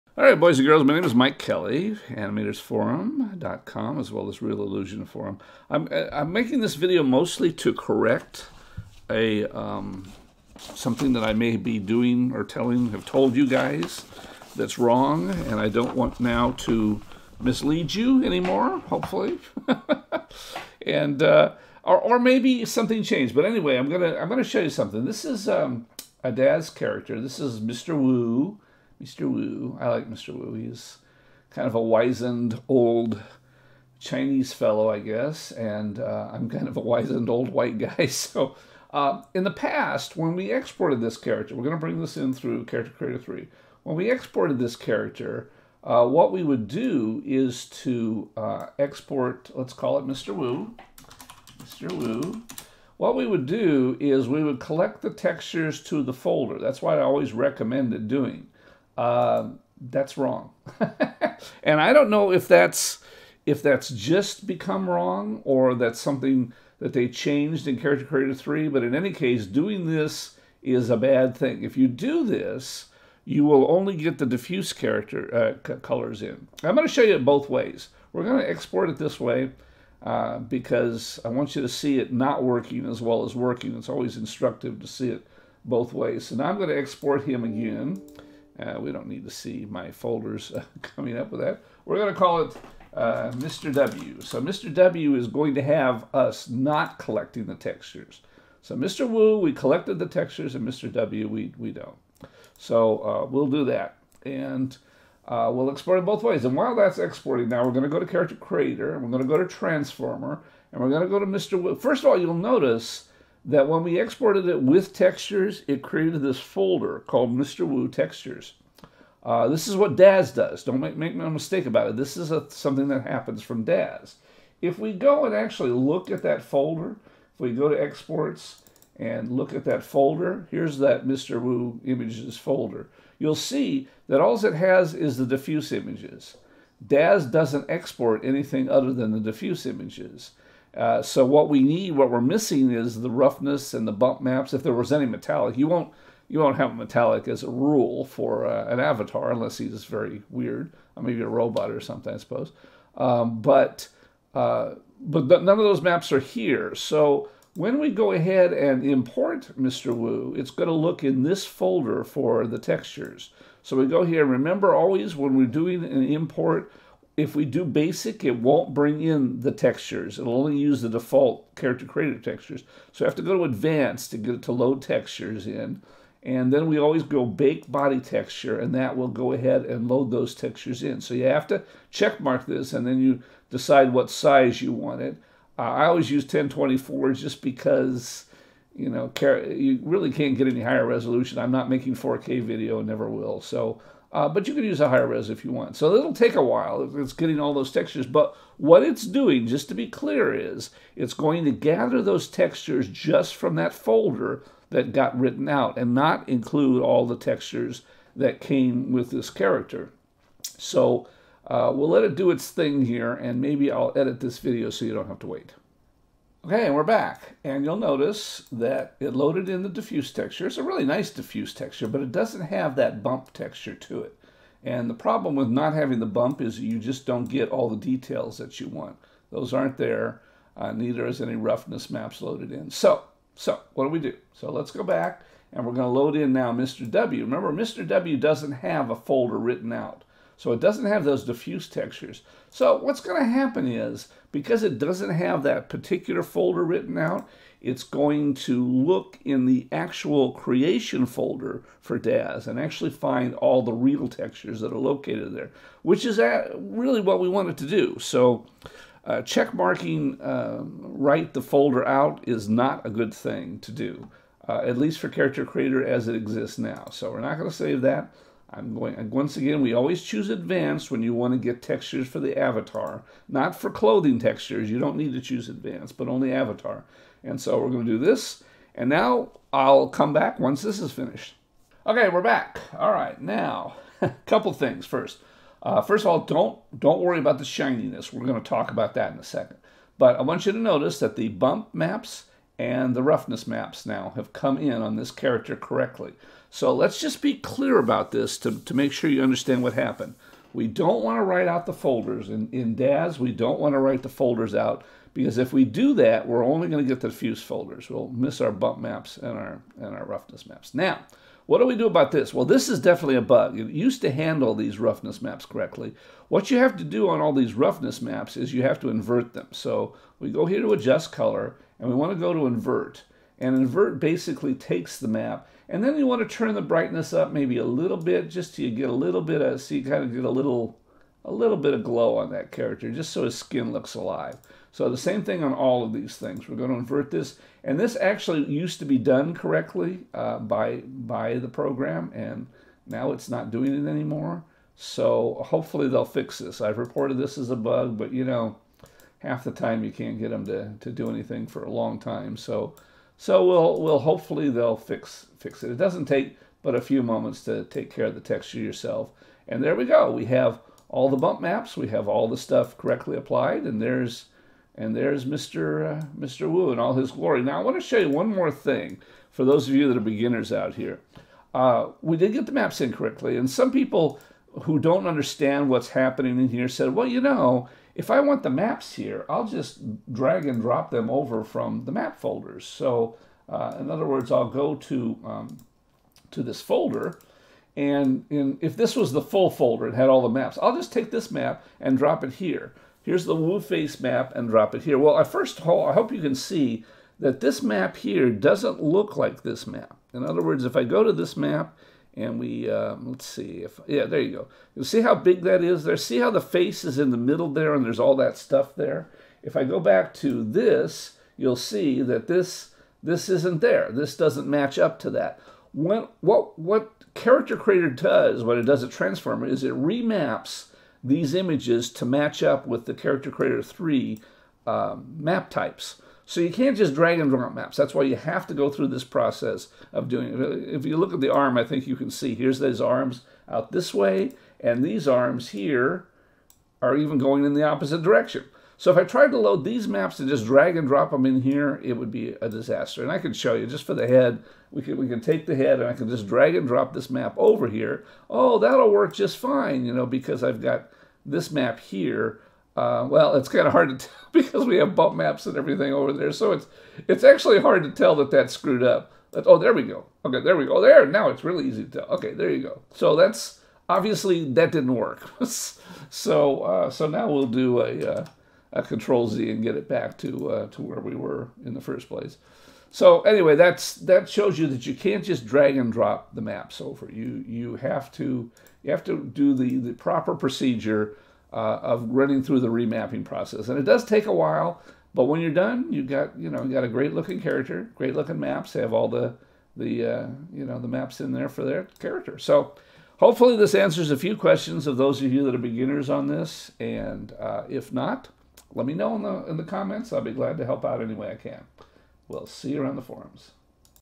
Convert Daz Characters to CC3 -- Skin complete textures
Showing how to get ALL the bump/normal/metallic/glossy maps into CC3 from Daz characters properly.